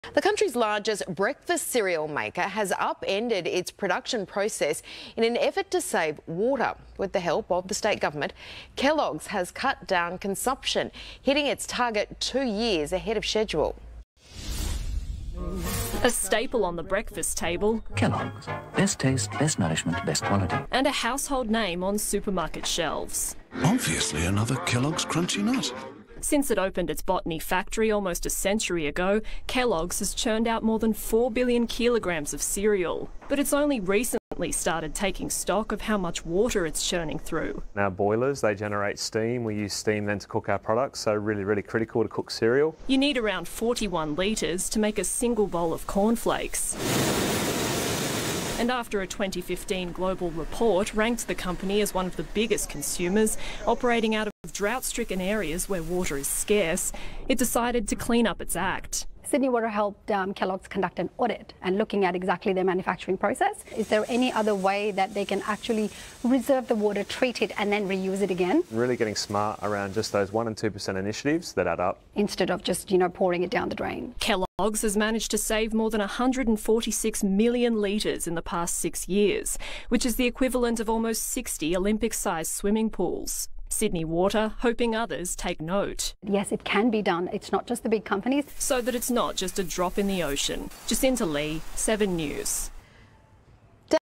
Kellogg's Better Days Promise - Down Under, conserving water in a 100-year-old factory
In 2015, Kellogg Australia embarked on one of its most ambitious projects at its almost century-old factory in Botany, near Sydney: reducing the amount of water we use—a key commitment of Kellogg’s Better Days® Promise to nurture our planet.

We partnered with Sydney Water, the municipal water provider for the city of Sydney and its suburbs, to see how it could be done. The results? We reduced water use by 43%, saving more than 146 million liters of water (almost 60 olympic-sized swimming pools!).